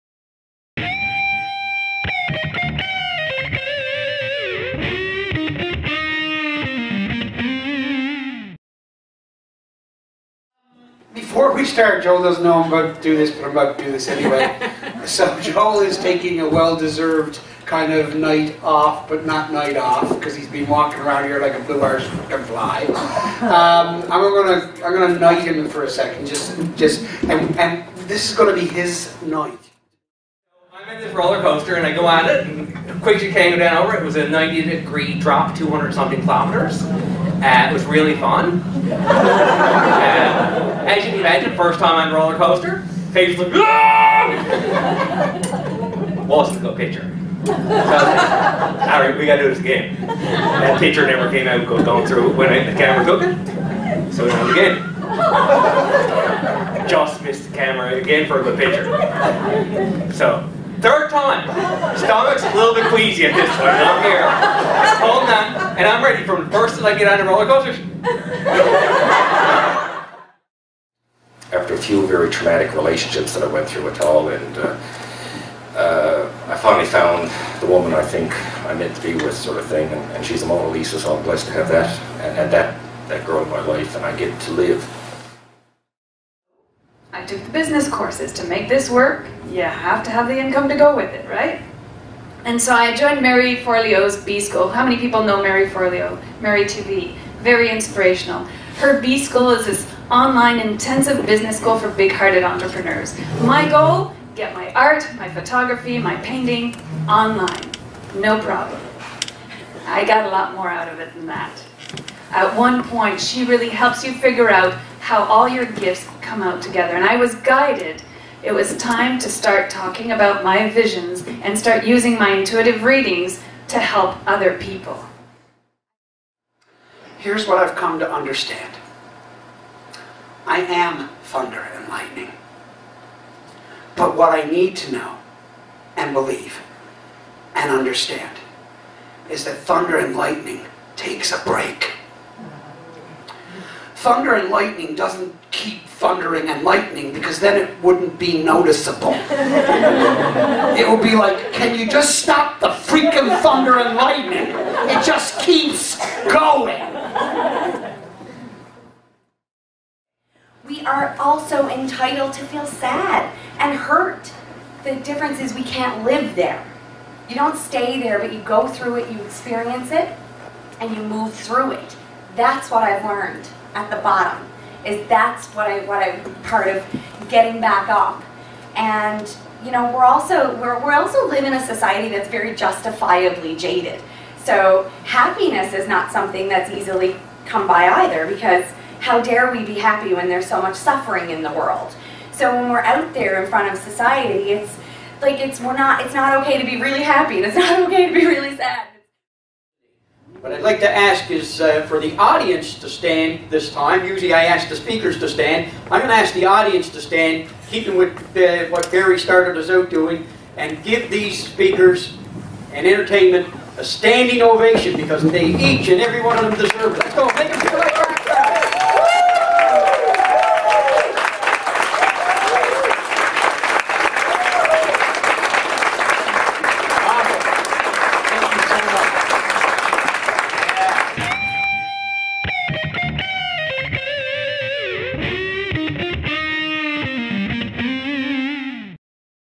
20160502 momondays St. John's
Welcome to momondays St John's. Your host is Joel Sweeney.

Speakers are:

Gerry Sullivan
Gerry has an energy and drive for life that can only be understood by hearing him. His perspective comes form his volunteer work with youth and fitness where he founded Pro Fit NL, a non-profit organization to help promote youth wellness. After overcoming his own health battles Gerry has lost over 150 pounds and now helps others achieve their goals, as well as working with companies in business growth.

Glenn Roil
Glenn Roil is a very active person in the community. He is a real lover of life. He sit on a number of boards and committee with the Government of NL and Eastern Health and with Community Agencies and Groups for Mental Health and Addictions. He is one the Public Faces for the Government of NL Anti Stigma Campaign for Mental Health and Addictions. He volunteers with the Janeway Telethon.

Dominique Hurley
Dominique Hurley is an intuitive artist and lightworker whose life purpose is to explore and express her love of beauty and the beauty of love. After a lifetime of following Guidance, she's helping others ignite their intuition through her paintings, photography, intuitive readings, guided meditations, workshops, and blog.

Barry Lewis Green
Barry is The Unity Guy™. EPIC Engage™ is his work; building strong, united and inspired cultures lead by character… where everyone matters and contributes. He engages on the global stage, doing just that. Barry speaks, dialogues, trains, facilitates, coaches, sings, dances, writes and cartoons his work as a Baha’i, entrepreneur, educator and Master Facilitator with The Virtues Project™.

Tonya Whittle
Tonya is a fitness mindset and weight loss strategist. She helps people understand why they don't/can't or won't so they can. Tonya understand that the weight isn't the problem, it's a manifestation of the problem. She helps you identify the issues and create new plans to move you forward making health and wellness a priority from a place of acceptance and being enough.

For information and tickets for the next momondays St John's or to apply to speak at momondays,